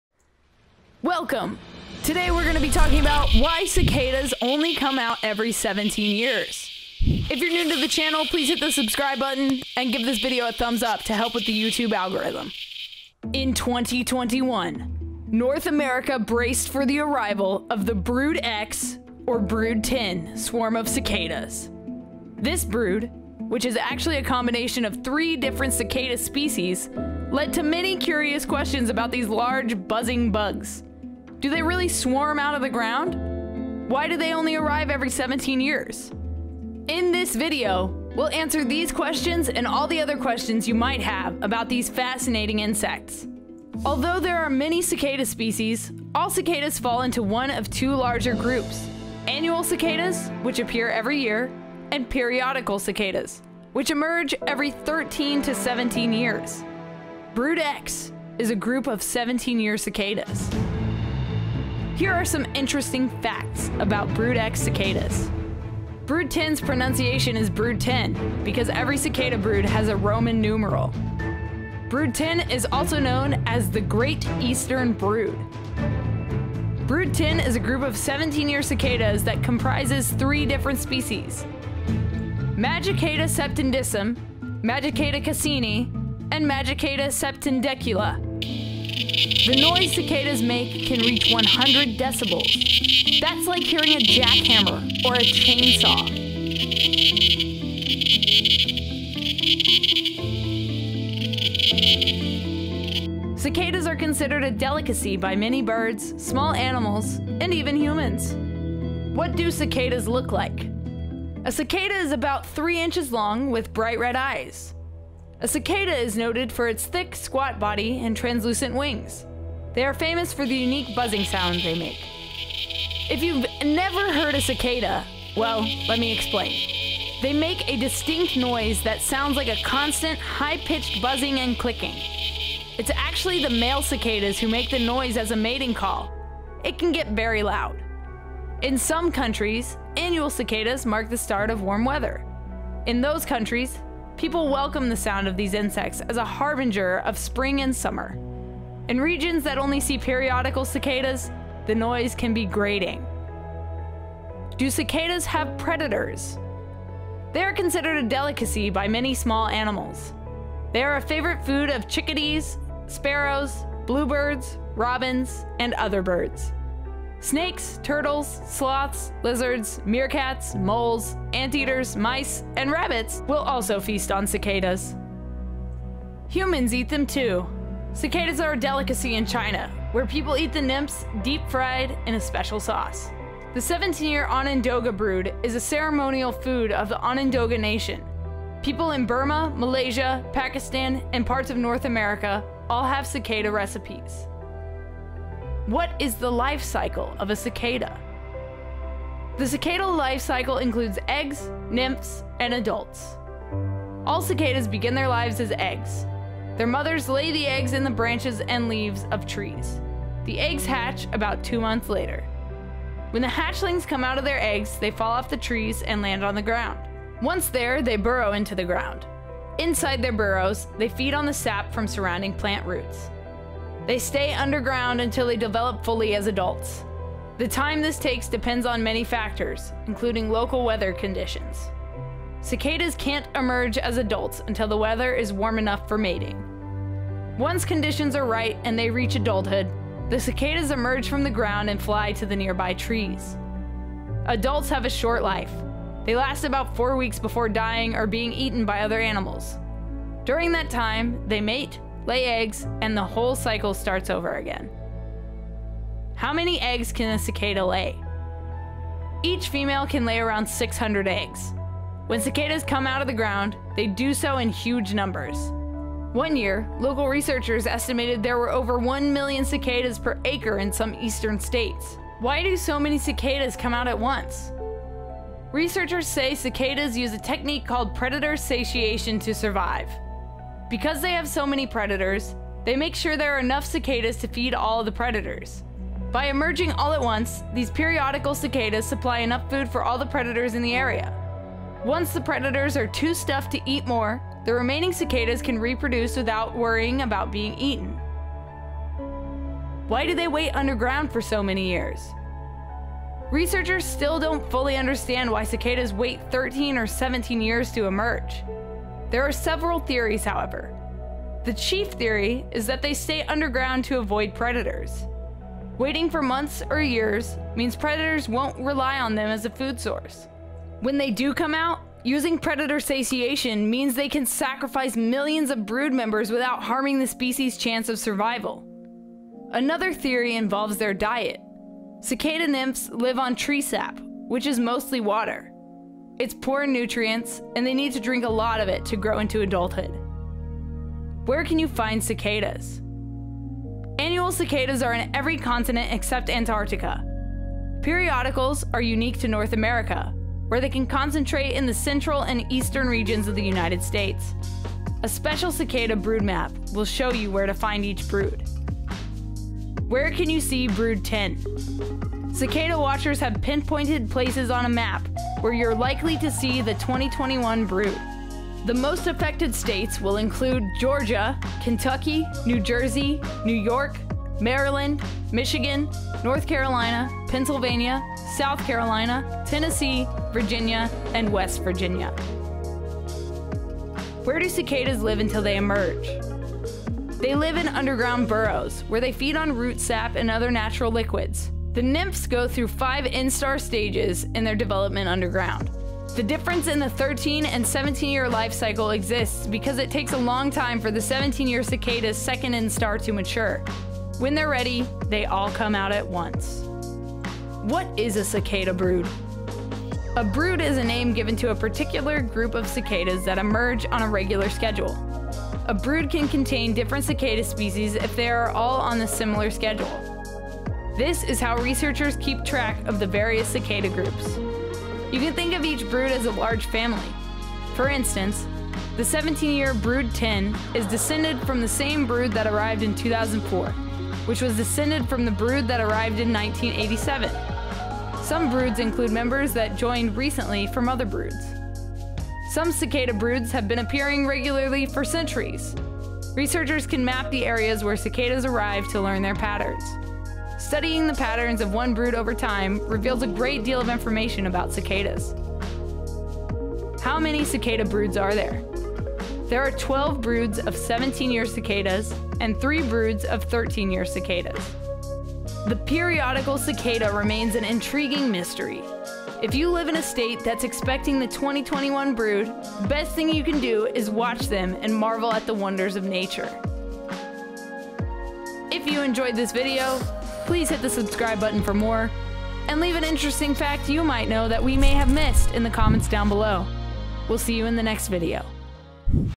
Why Do Cicadas Only Come Out Every 17 Years?
Learn more about why cicadas come out every 17 years

Although there are many cicada species, all cicadas fall into one of two larger groups: annual cicadas, which appear every year, and periodical cicadas, which emerge every 13 to 17 years. Brood X is a group of 17-year cicadas.